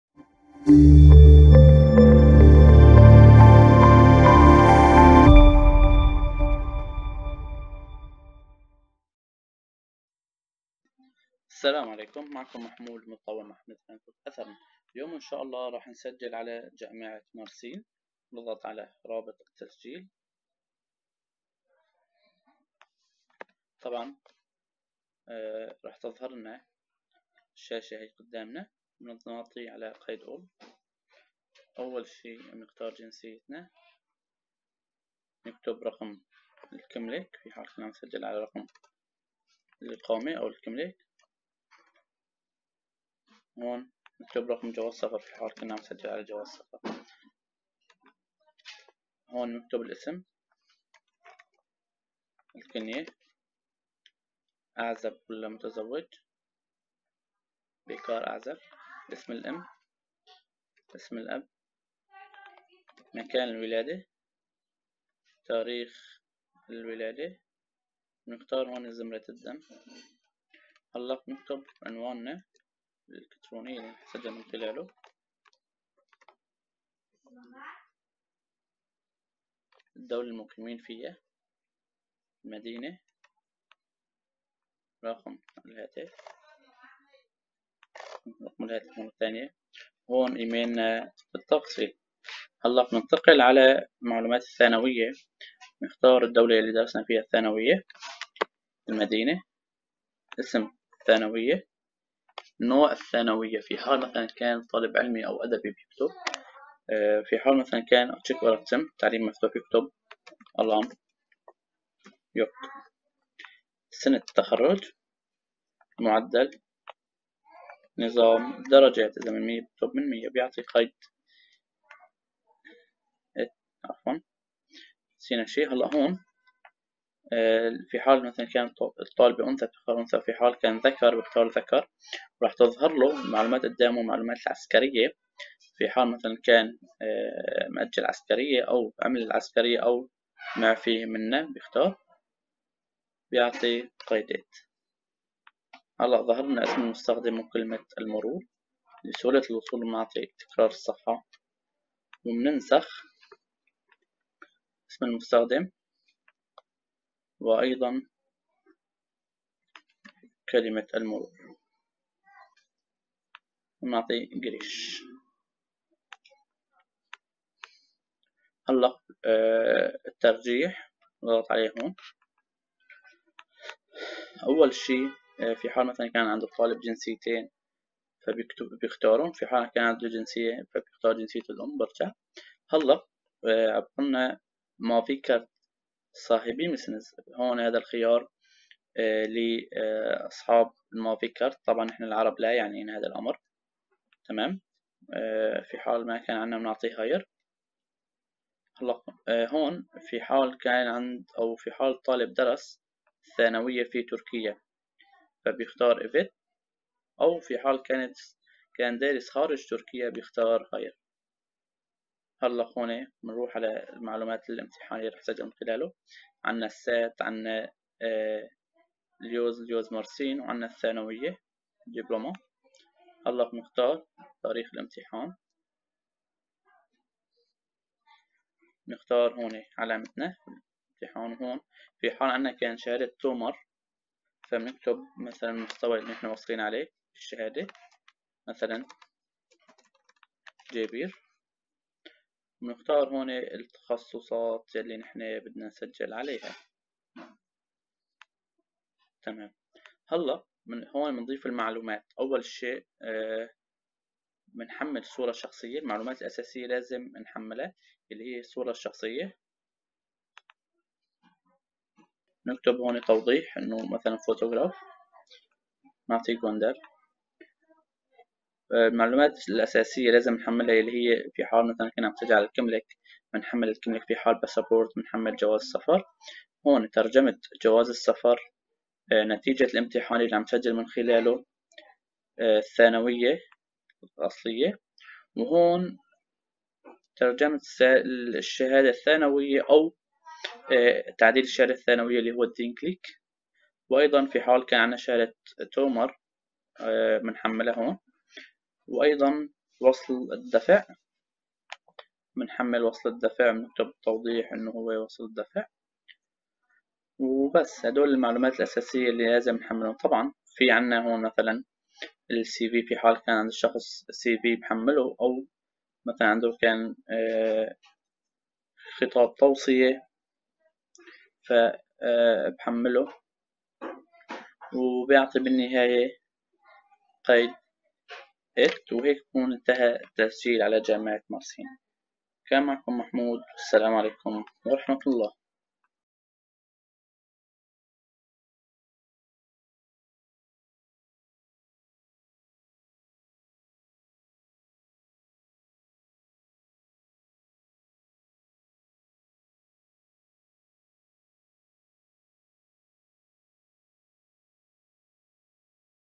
كيفية التسجيل على مفاضلة جامعة مرسين 2021
وايضا معلومات عن امتحانات اليوس واسئلة سنوات سابقة اضافة  لفكر جديده وملفات هامة لطلاب اليوس عن طريق موقعها إلكتروني تجدونهم هنا :